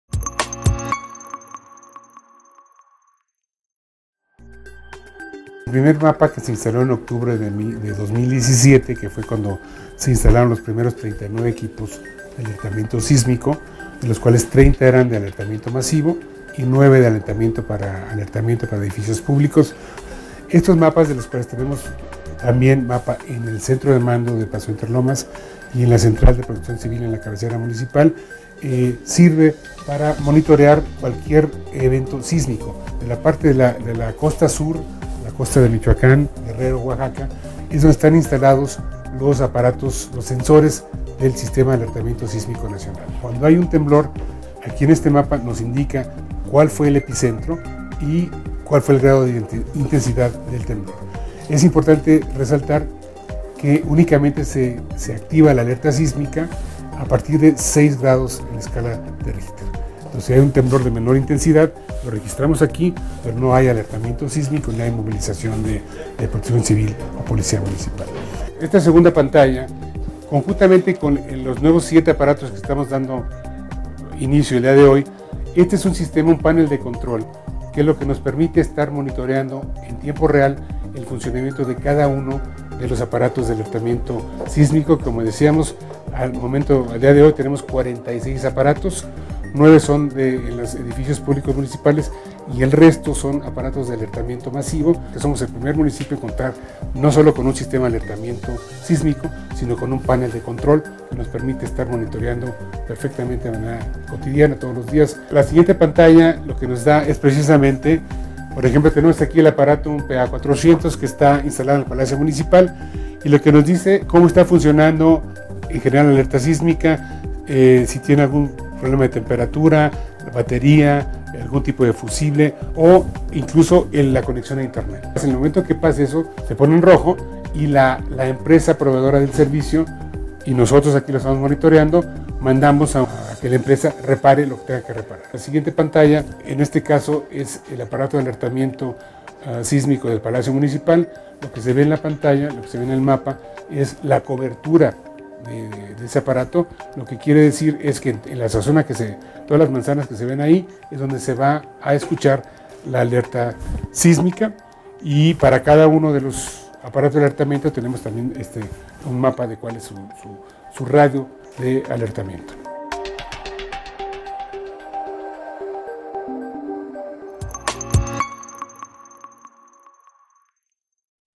1er municipio del Edo. Mex con sistema de alertamiento masivo sísmico
Hoy avanzamos en la protección de las familias huixquiluquenses, al convertirnos en el 1er. municipio en Edomex en contar con un sistema de alertamiento masivo sísmico y un panel de control que garantiza su funcionalidad. Mi reconocimiento a la Secretaría Técnica por este gran paso.